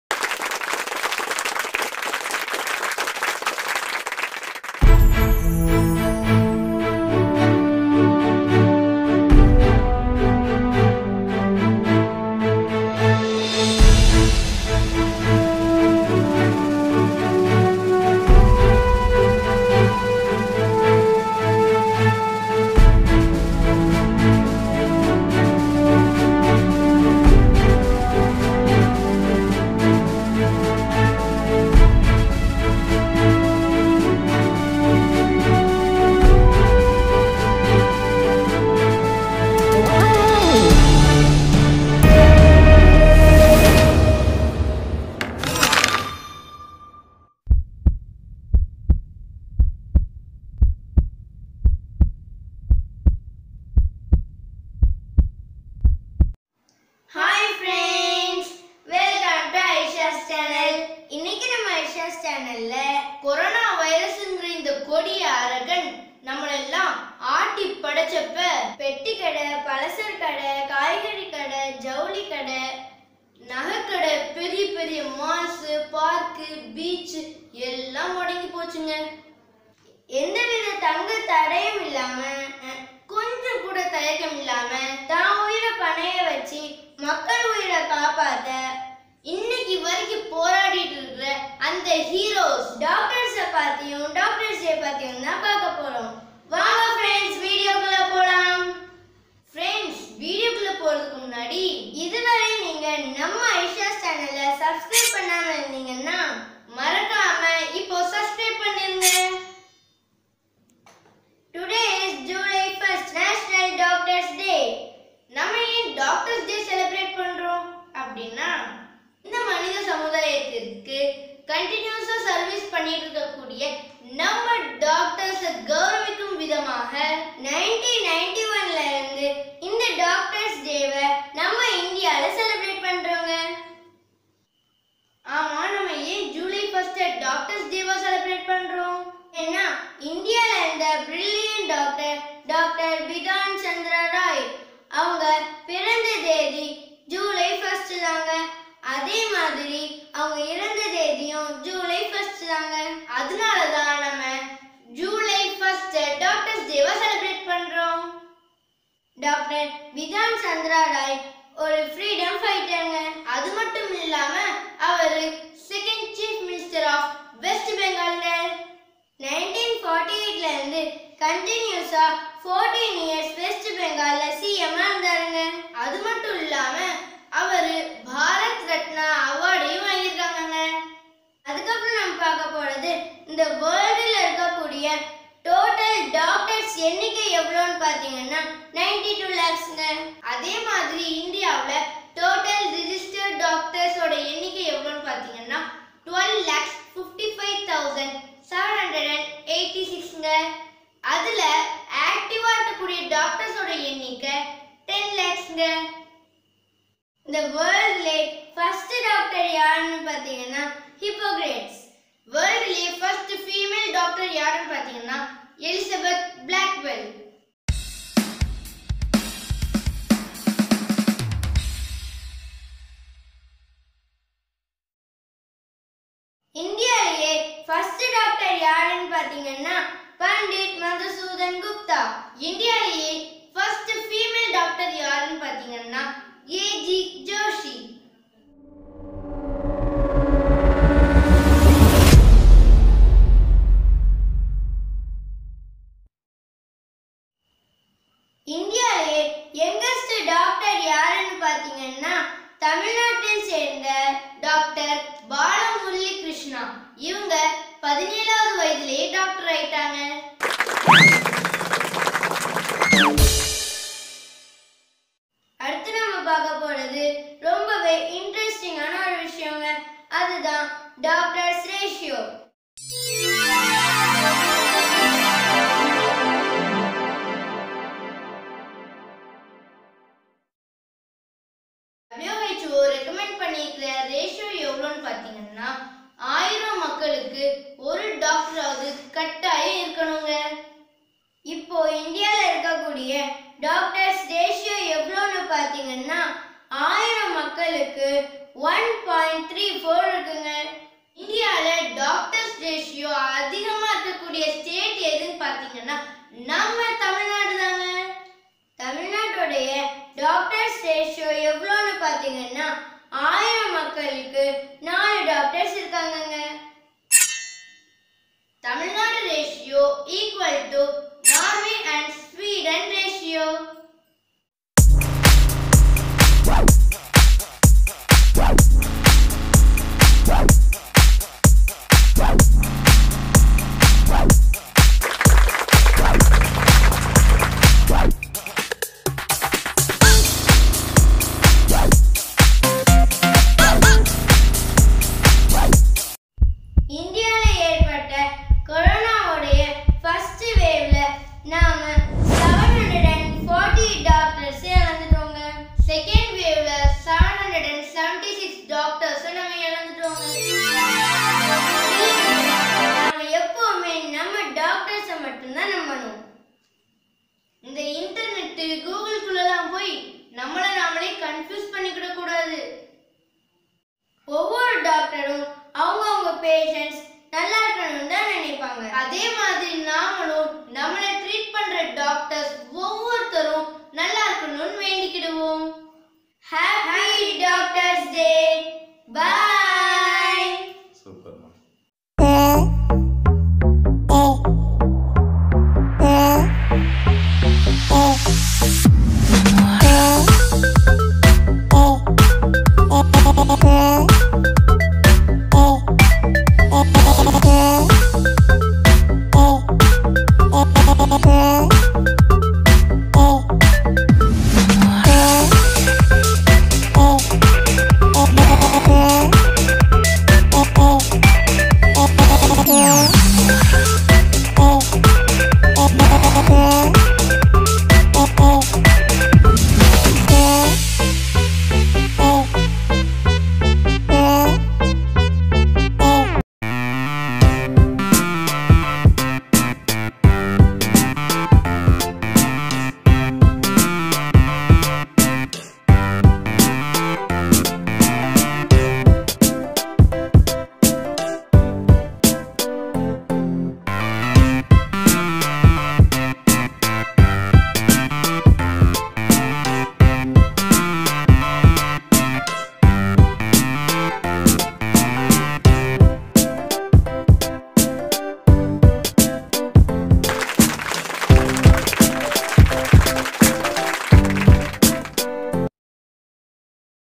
HAPPY DOCTORS DAY 2021 தேசிய மருத்துவர்கள் தினம் 2021
TO ACKNOWLEDGE THE CONTRIBUTION OF HONOURABLE DOCTORS TOWARDS THIS SOCIETY, WE CELEBRATING THIS SPECIAL DAY

"DOCTOR'S DAY" IN INDIA SINCE 1991.